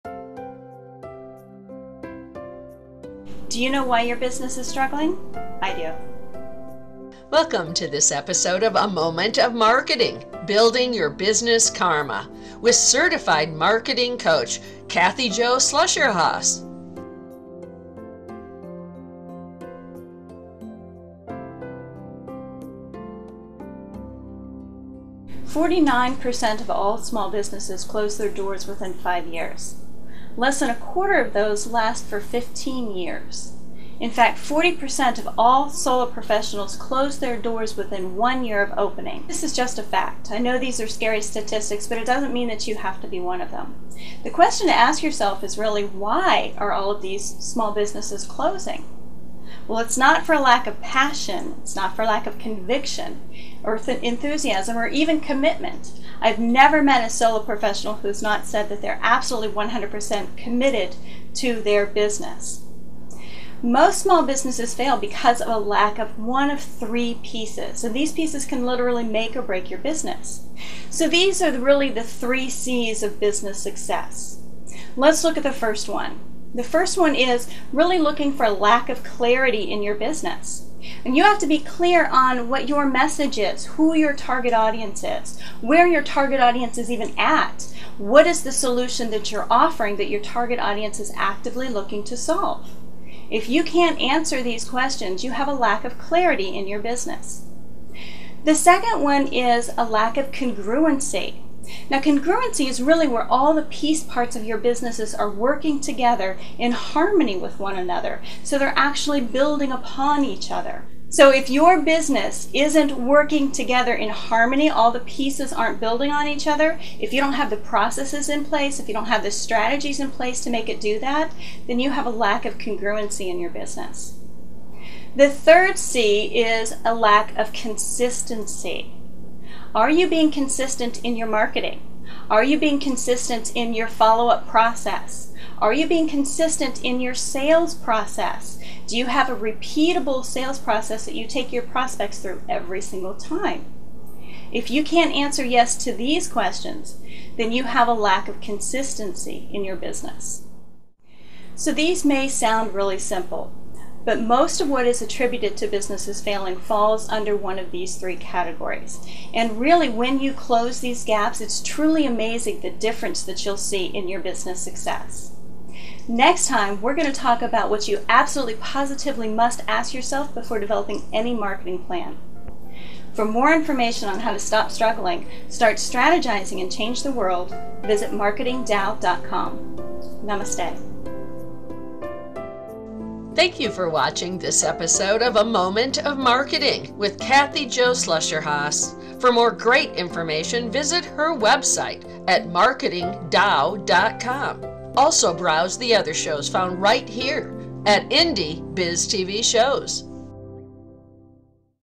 3 Cs of successful business
Learn from Kathy Jo Slusher-Haas the 3 C's of building a successful business